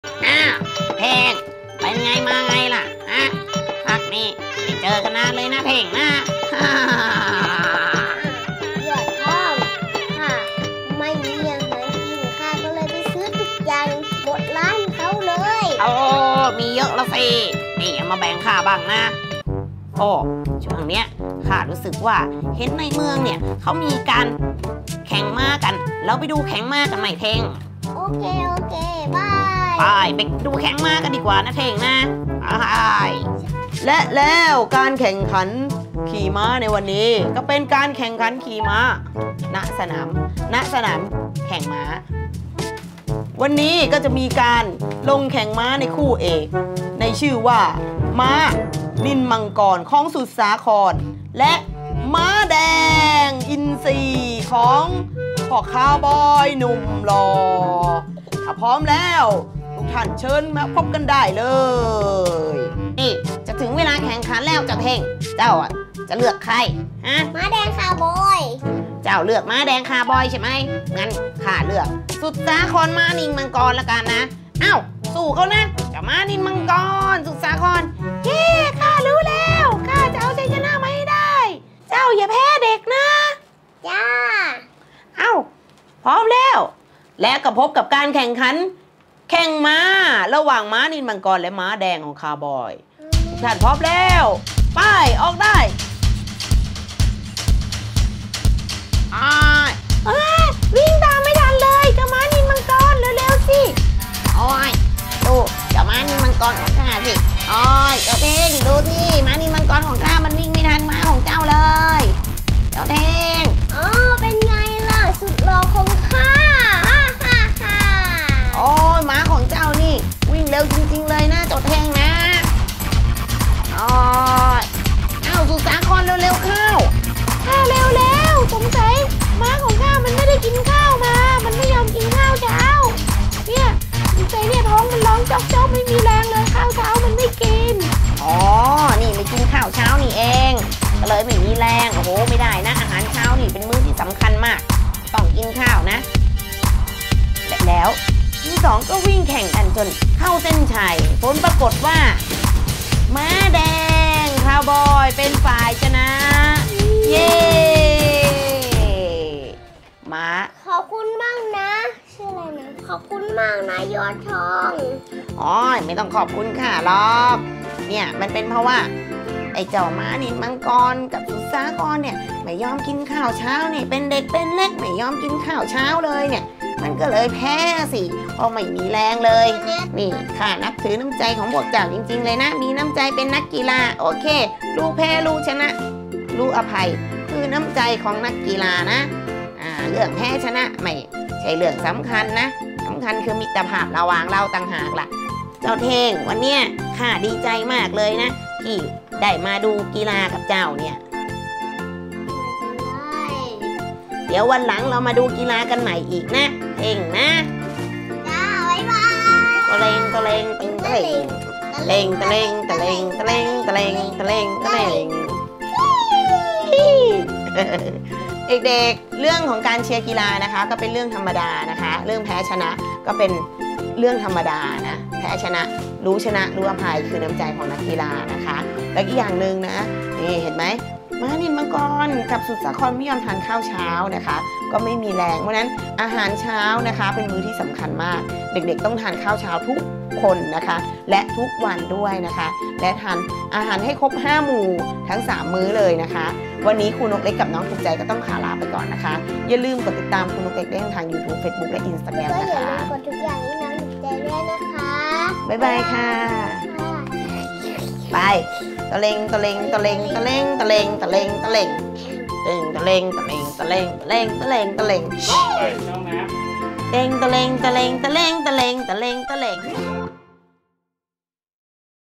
น้องถูกใจ | ตะลุงสุดสาครแข่งม้า
ไปดูครูนกเล็กกับน้องถูกใจเล่นหนังตะลุงกันดีกว่า